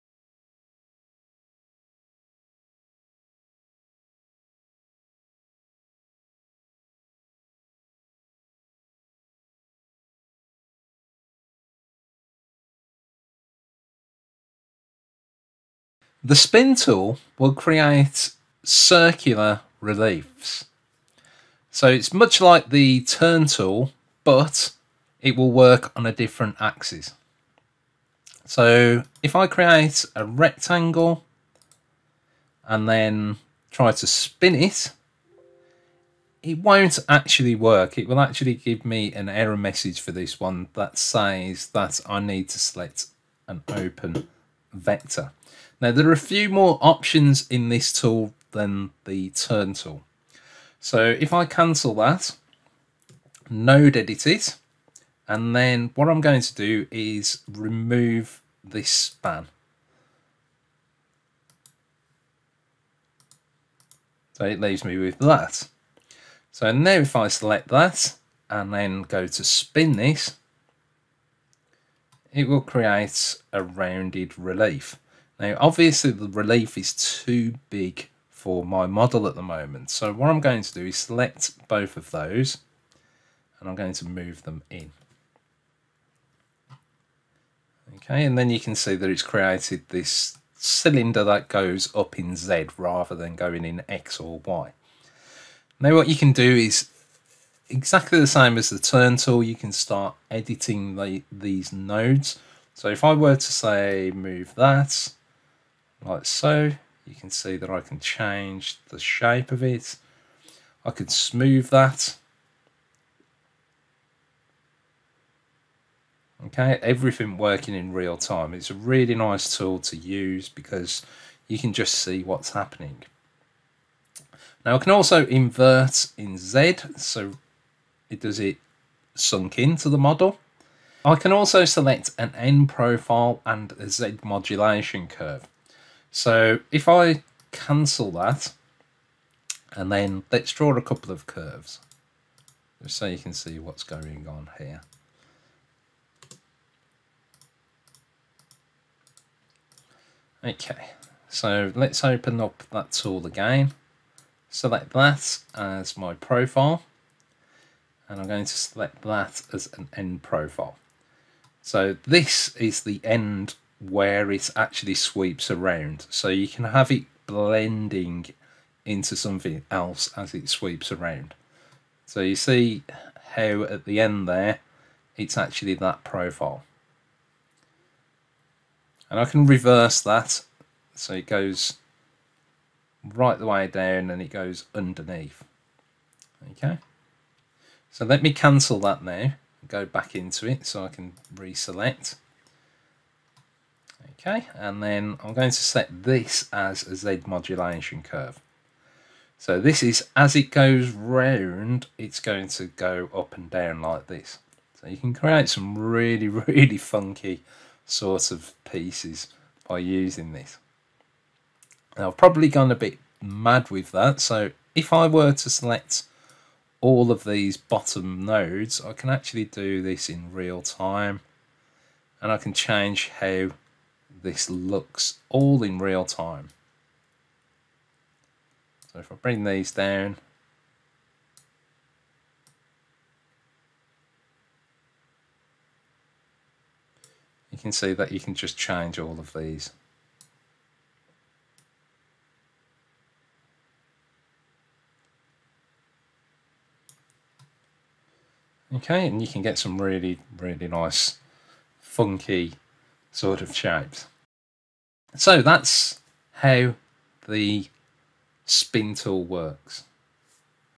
Carveco Maker Plus: Introducing the Spin Tool
Here Leighton shows you everything you need to know about the Spin Tool available on our Maker Plus and Carveco software. It's a jam-packed lesson that will give you all the knowledge you need to create interesting and funky designs unique to you!

Bookmarks:
00:00 - Introduction
00:29 - Spin Tool
00:50 - Spinning an Open Vector
01:15 - Resizing the Relief
01:35 - Editing in Real Time
02:03 - Inverting in Z
02:10 - Selecting an End Profile
03:14 - Setting a Z Modulation Curve